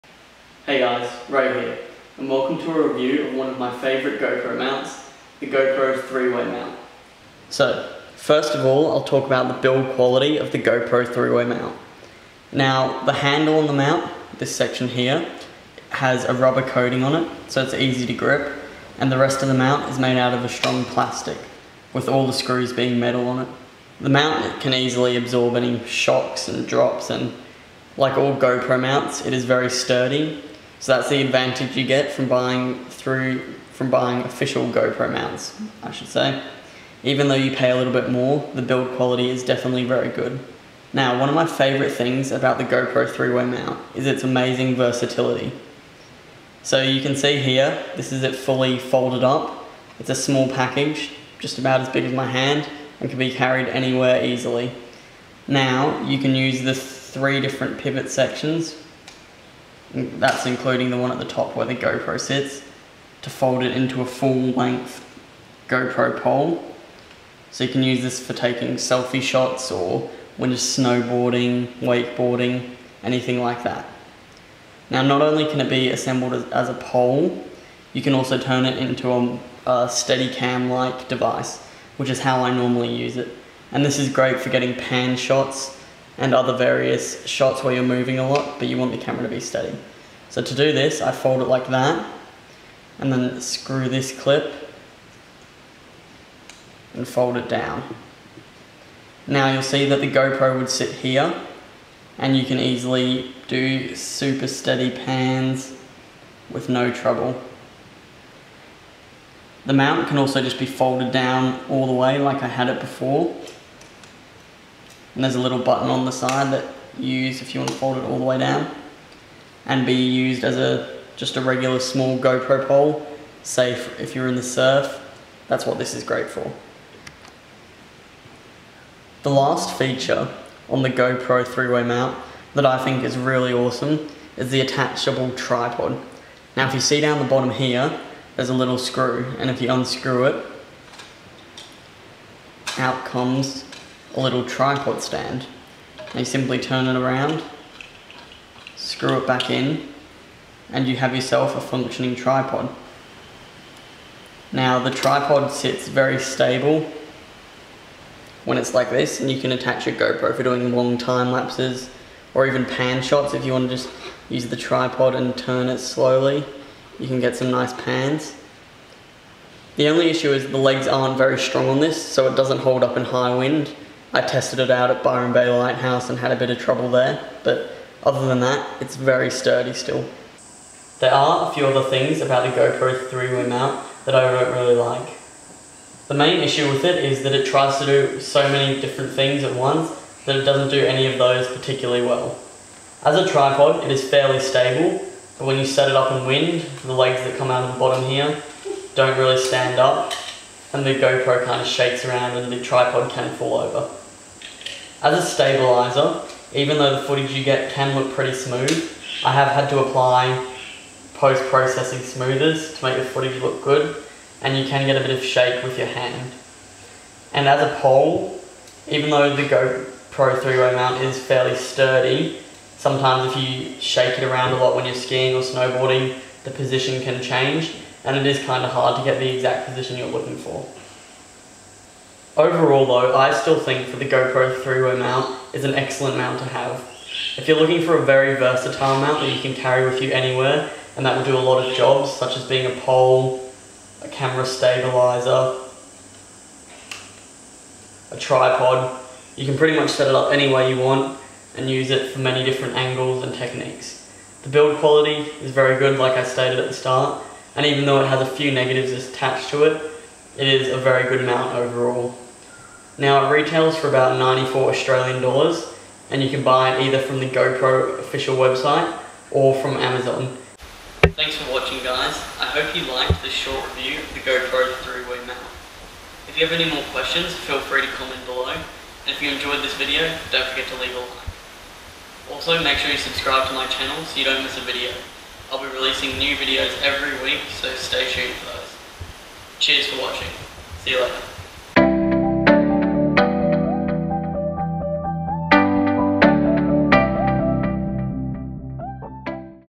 GoPro 3 Way Mount Review
A quick review of one of my favourite GoPro Mounts - The GoPro 3 Way Mount. The 3 Way mount is very versatile and gives you the ability to shoot many different angles and videos. It is one of my favourite mounts because, as stated in the review, it has many features packed into a small but sturdy form factor. There are some things I dont really like about the GoPro 3 Way mount - the fact that even though it can be used as many different mounts, it doesnt really do any of its jobs particularly well is the main negative. Overall this is a great mount and for $94 AUD you get a very sturdy durable piece of equipment.